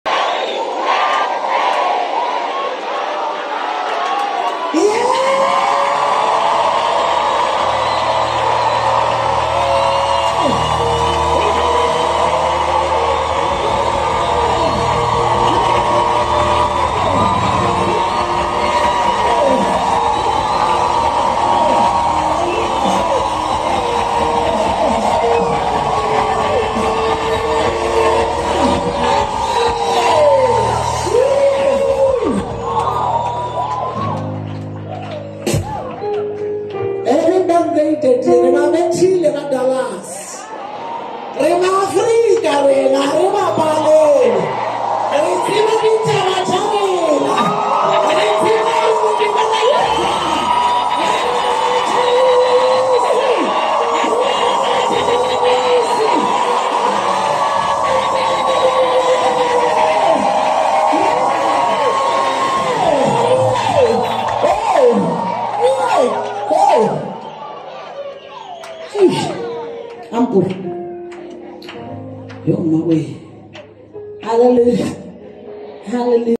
𝐌𝐨𝐦𝐦𝐲 𝐚𝐧𝐝 𝐭𝐡𝐞 𝐜𝐨𝐧𝐠𝐫𝐞𝐠𝐚𝐭𝐢𝐨𝐧 𝐰𝐢𝐬𝐡𝐢𝐧𝐠 𝐃𝐚𝐝𝐝𝐲, 𝐀𝐩𝐨𝐬𝐭𝐥𝐞 𝐁𝐉 𝐌𝐚𝐤𝐚𝐧𝐚𝐧𝐢𝐬𝐚 𝐚 𝐇𝐚𝐩𝐩𝐲 𝐁𝐢𝐫𝐭𝐡𝐝𝐚𝐲
We are blessed to have you in our generation.